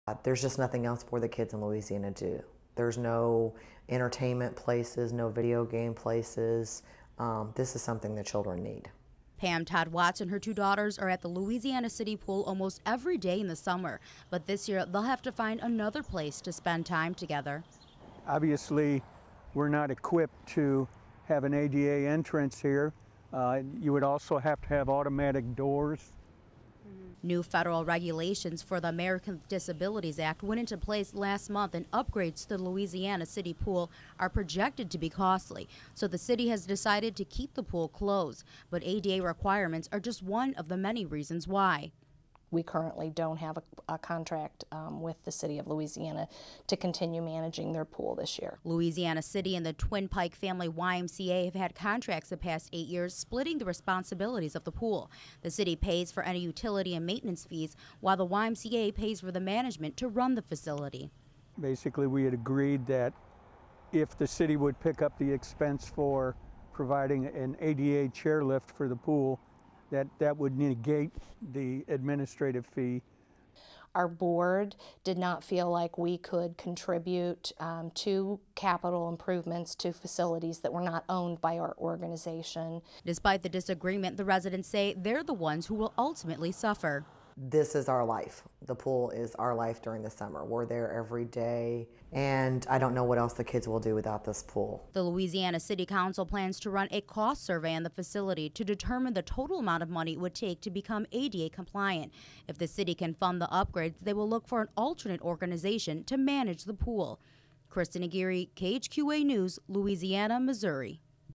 A dry summer for Louisiana residents
A local pool is forced to keep its doors closed this summer and residents are worried how the dry season will effect the community.

"There's just nothing else for the kids in Louisiana to do," Pam Todd-Watts, a Louisiana City resident said. "There's no entertainment places, there's no video game places, this is something the children need."